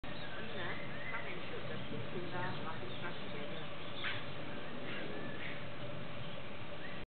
Chestnut-headed Laughingthrush
Always the clown @ Kinabalu National Park, Sabah